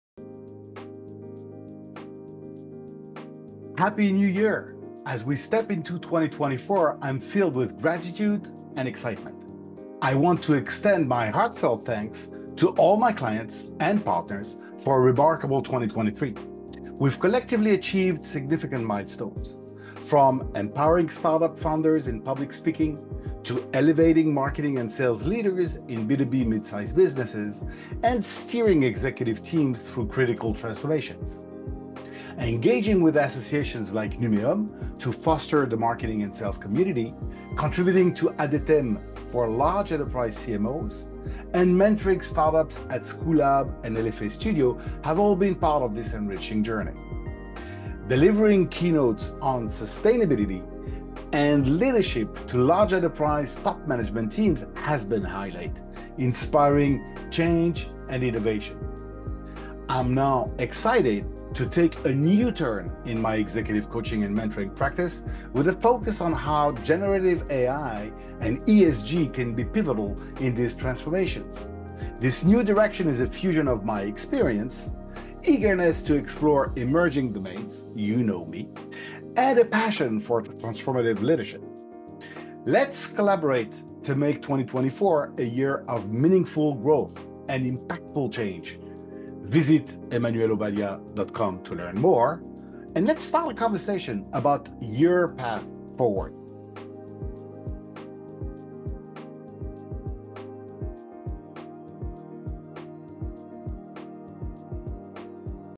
Emmanuel Obadia's Happy New Year 2024: Leading Change with AI & ESG
Join me, Emmanuel Obadia, as we step into the promising year of 2024. In this special New Year’s message, I express my deep gratitude to all my clients and partners for their invaluable support and collaboration throughout a transformative 2023.

In this brief video, I reflect on the year gone by, highlighting significant achievements such as empowering startup founders to master public speaking, assisting marketing and sales leaders in mid-size B2B businesses, and guiding executive teams through pivotal changes. I also share my enriching experiences with Numeum, ADETEM, Schoolab, and LFA studio, which have significantly contributed to my professional journey.

An exciting highlight of 2023 was delivering inspiring keynotes on sustainability and leadership, igniting change and innovation among top management teams in large enterprises.

As we look forward to 2024, I’m thrilled to announce a new direction in my executive coaching and mentoring practice, focusing on leveraging Generative AI and ESG for transformative leadership and business growth. This new venture represents a blend of my accumulated experience, passion for exploring new horizons, and commitment to transformative leadership.

Join me in this new chapter. Let's make 2024 a year of impactful collaboration and growth. Visit emmanuelobadia.com to learn more and engage in a conversation about your journey towards transformation and success.